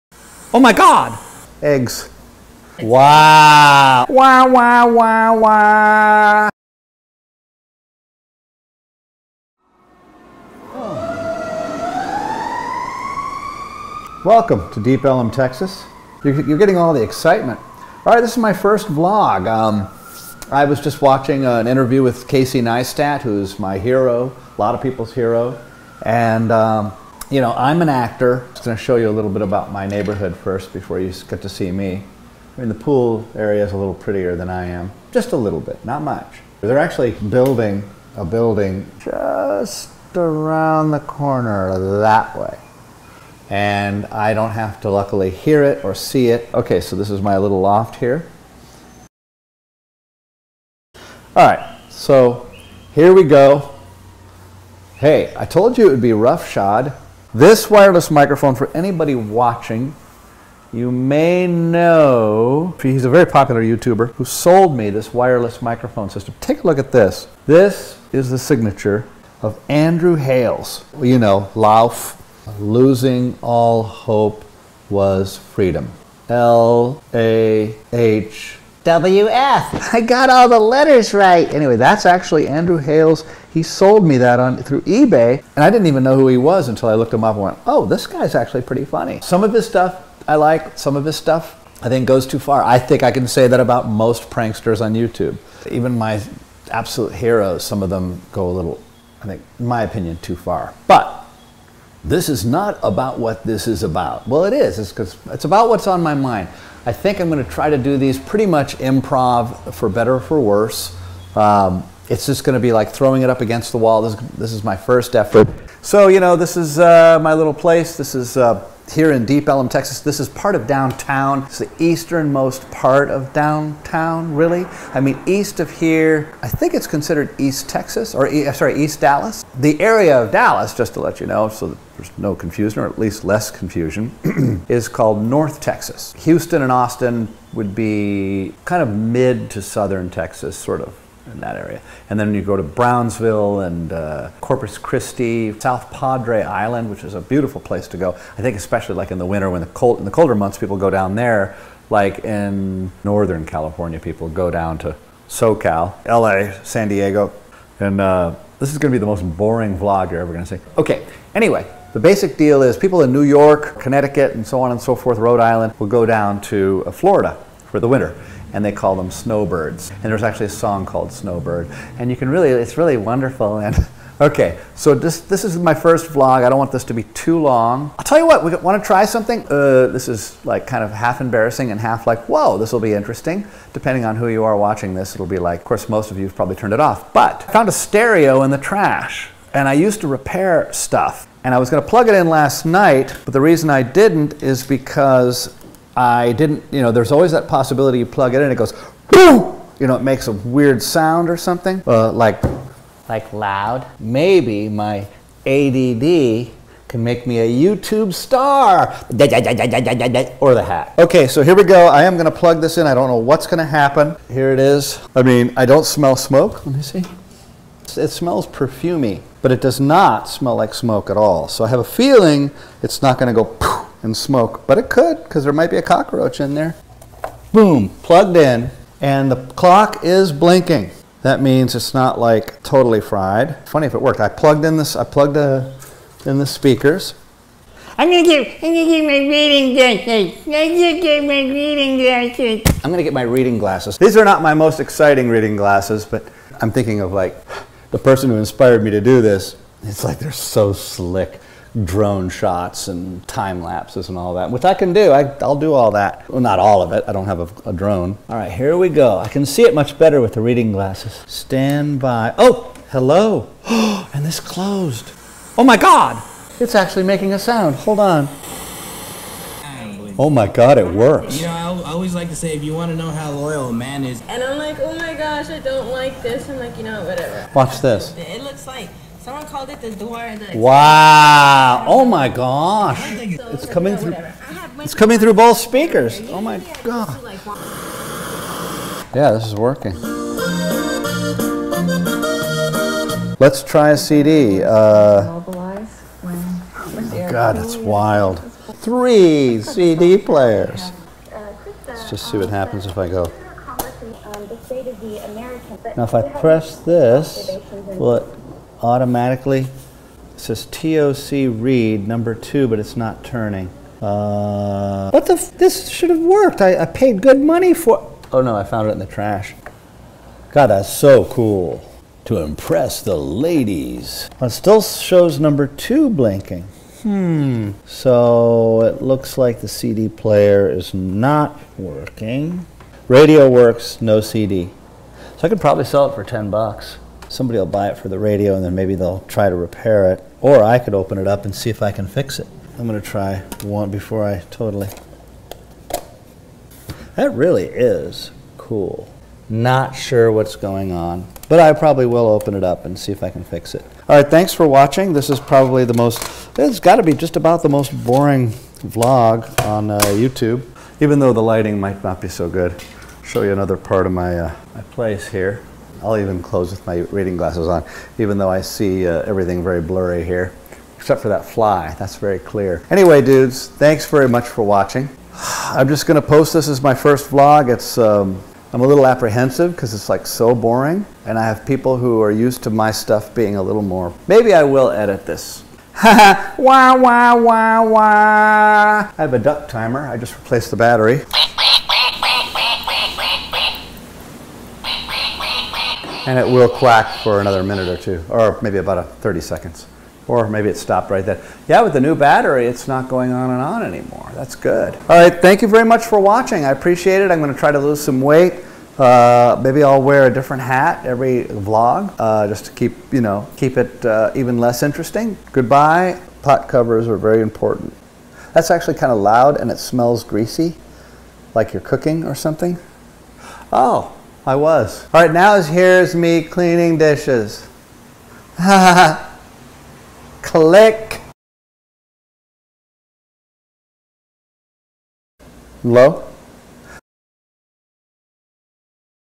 I Found it in the Trash!! --- Ari's Vlog #1 - b e g i n
This is the 1st of at least 7 daily vlogs.  It might be super boring, but at least it's a 1st!